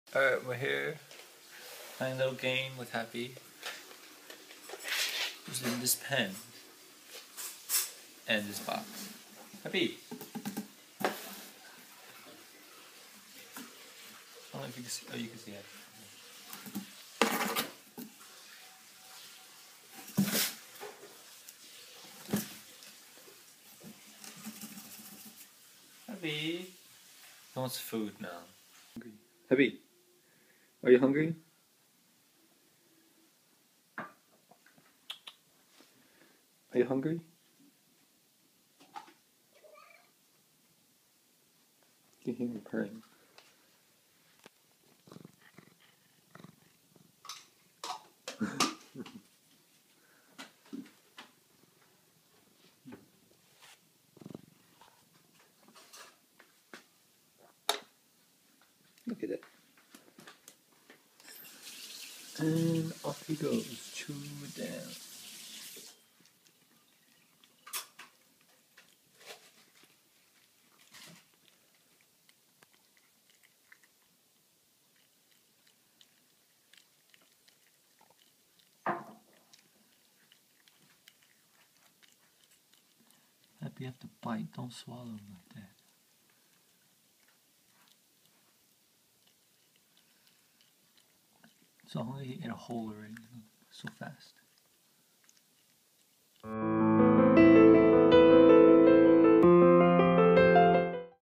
Vegetarian cat chooses food over games! [HD]
This is Happy, my cat! He's been a Vegetarian his whole life and decides to eat instead of play. Enjoy!